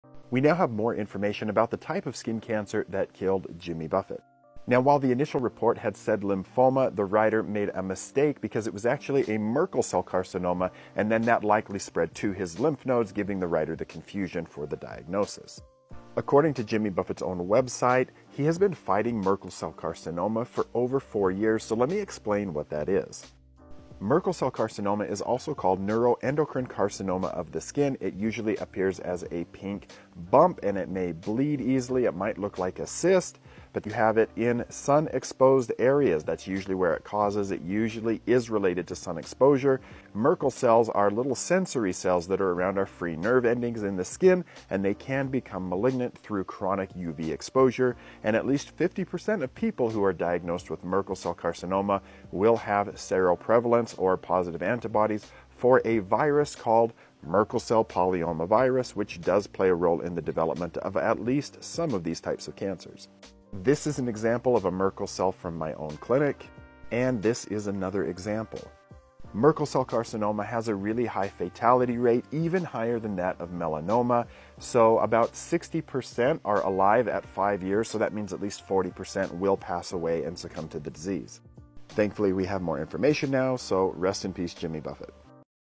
Dermatologist Explains - What you Need to Know About Merkel Cell Carcinoma
Legendary singer, Jimmy Buffett recently passed away, and it has been disclosed that he has battled Merkel cell carcinoma for the past four years

Merkel cell carcinoma is an incredibly aggressive and rare form of skin cancer. That usually appears in sun exposed areas. ￼￼

As a dermatologist, I have treated a handful of patients with Merkel cell carcinoma. In this video, I discussed some of the risk factors for Merkel cell carcinoma, and it’s not just sun exposure.

Be sure to leave any questions in the comments of this video, and I will answer them